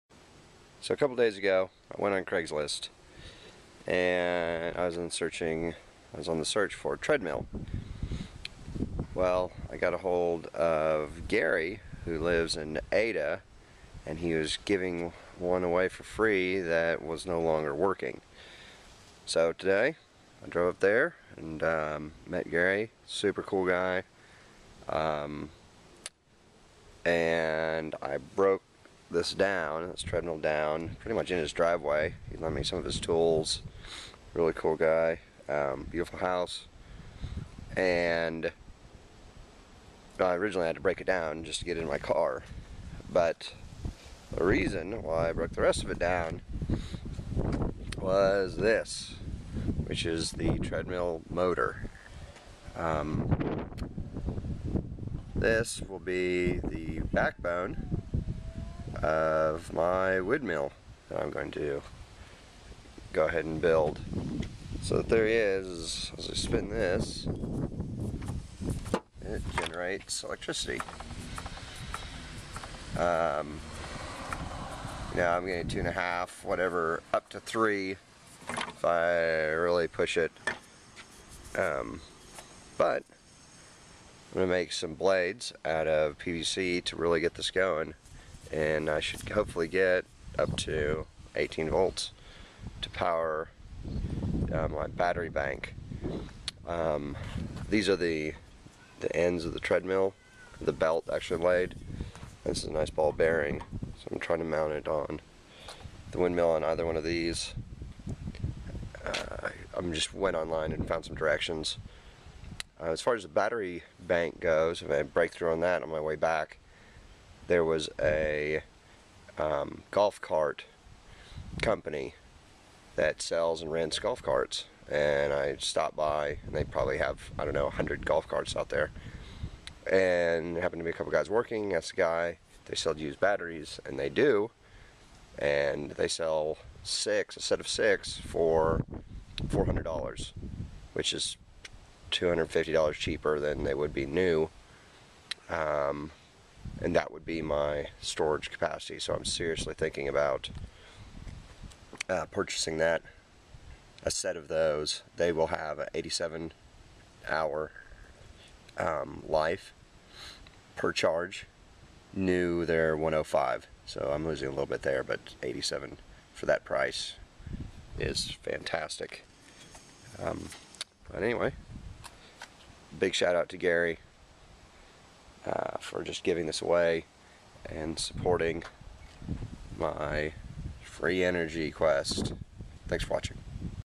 Wind Turbine: Part 1 Finding the motor
With the generosity of Gary, I procure my first treadmill motor to be used as a wind turbine.